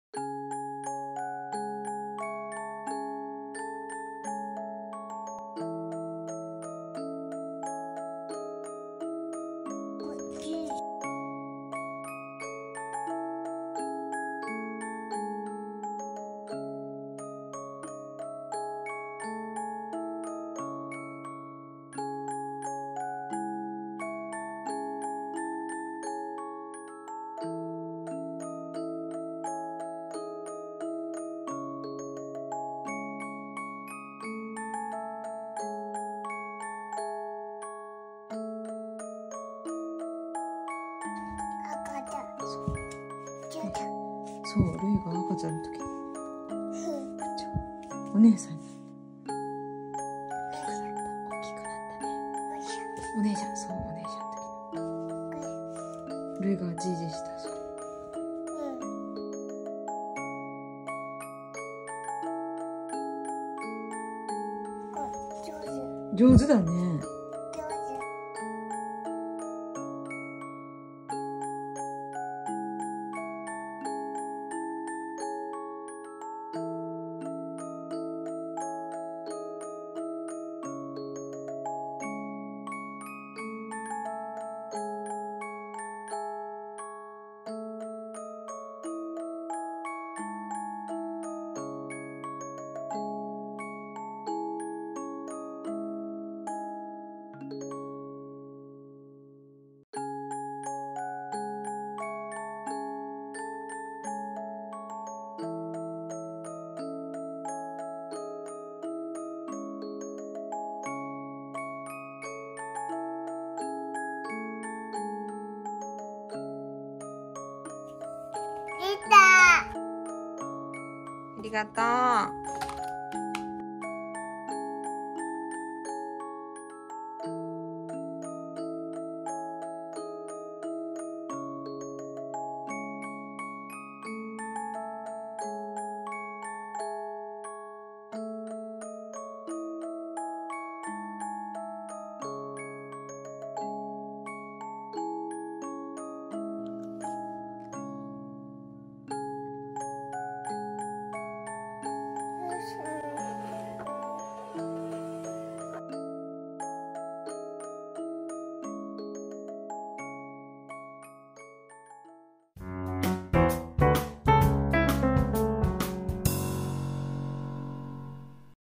お雛様作品大量生産🎎！ 1年間で成長した姿に感動🥹❤️@ガジガジるいチャン
こんにちは😃
これからもおもしろ可愛い るい の日常をお届けしますので、ぜひ見てくださいね😊🙌

子育て支援センター行きまくってるおかげで、お雛様作品が大量にできました🎎⸝⋆
自分の作品をせっせとリメイクして遊んでます🙄

ひな祭りメニューは、可愛いものが作れる自信がなかったので（ママ、手抜き）3色団子もどきを作りました🍡
一口も食べずにポイ！予想通りの反応だったので、ママの心はノーダメージでした🫠笑

これからも元気いっぱいで、おおきくなってね🤗❤️